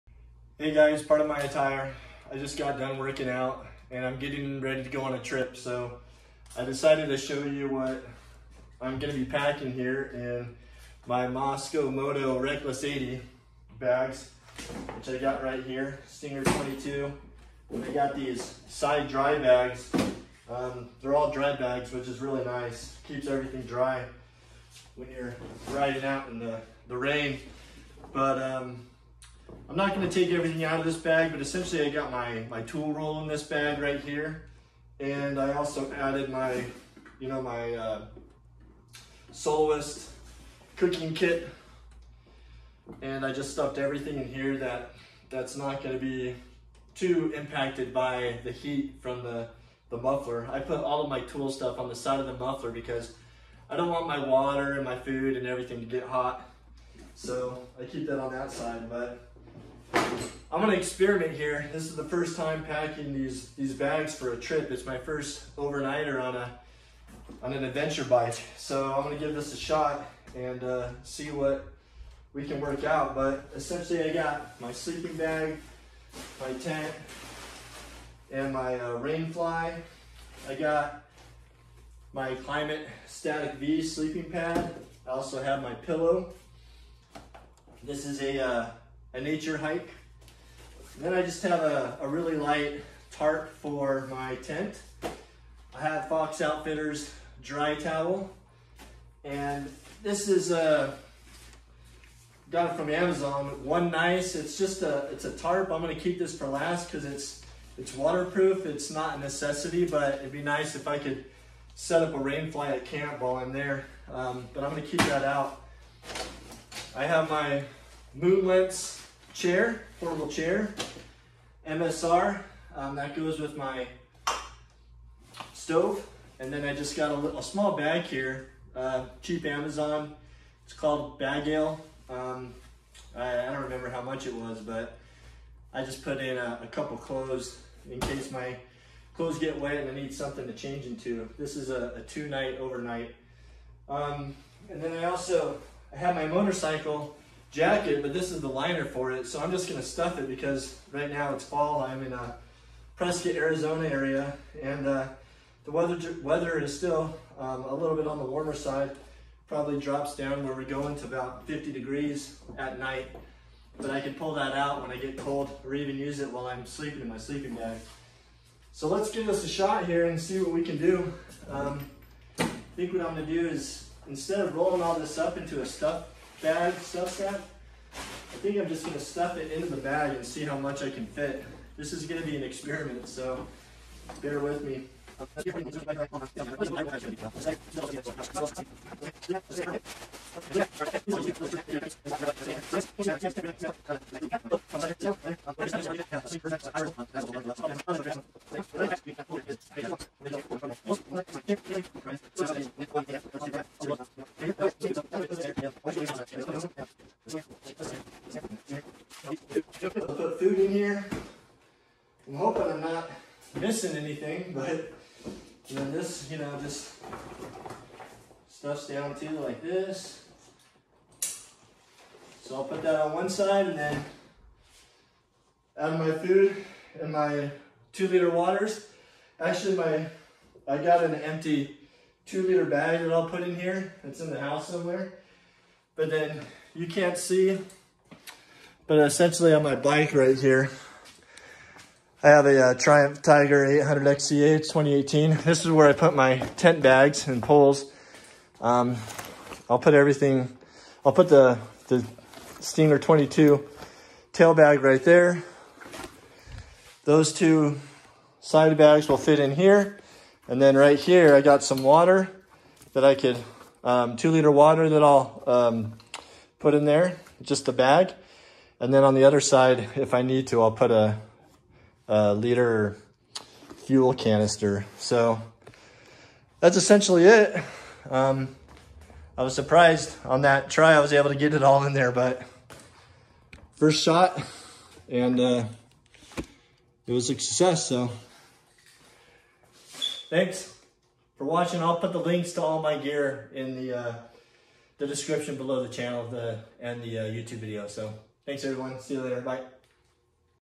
Packing Guide: Discover What I Packed on My Triumph Tiger 800 for a campout
Welcome to my packing guide for a recent campout from my Triumph Tiger 800 motorcycle. In this video, I'll show you exactly what I packed on my motorcycle for this exciting campout trip.

Join me as I detail the essential items and gear specifically chosen for the Tiger 800. From camping necessities like tents, sleeping bags, and cooking equipment, to adventure-specific gear such as tools, and riding accessories.

Discover my tips and tricks for optimizing your packing space on the Tiger 800, ensuring a comfortable and unforgettable adventure. Learn about the various storage options with the Mosko Moto Reckless 80L to see how I strategically organize my gear for easy access during the trip.